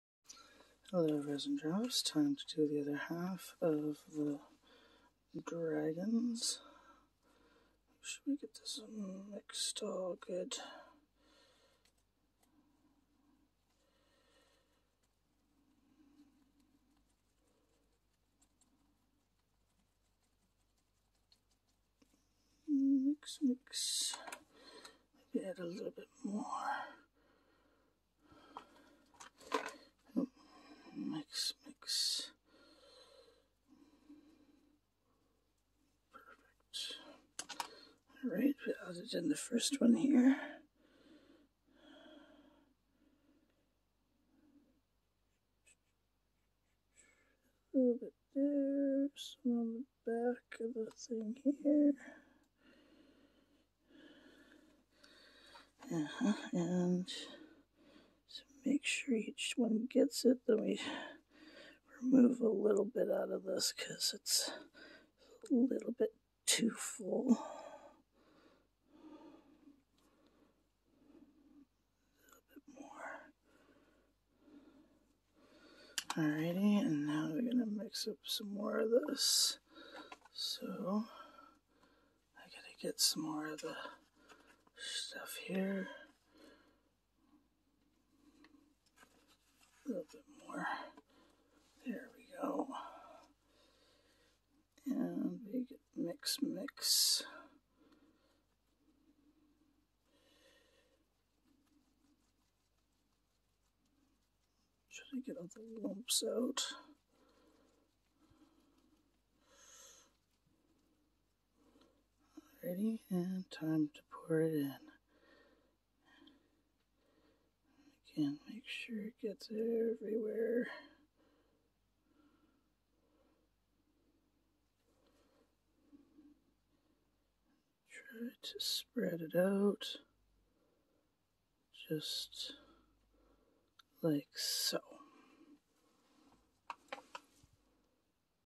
Adding resin to other half of baby dragons
Adding white mica powder in resin to add to the other half to baby dragons.